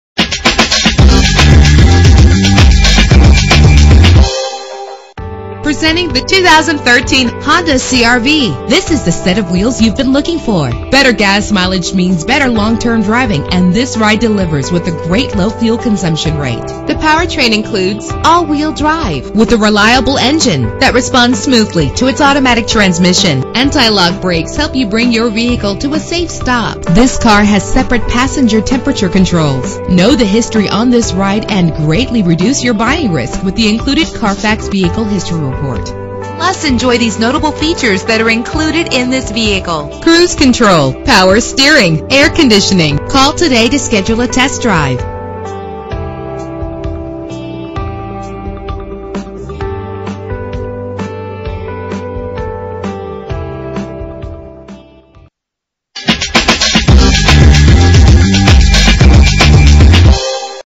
2013 Honda CR-V - Little Falls NJ
Year: 2013
Make: Honda
Model: CR-V
Trim: LX
Engine: 2.4 L, 4 cylinder
Transmission:
Color: Red
Mileage: 29472
Address: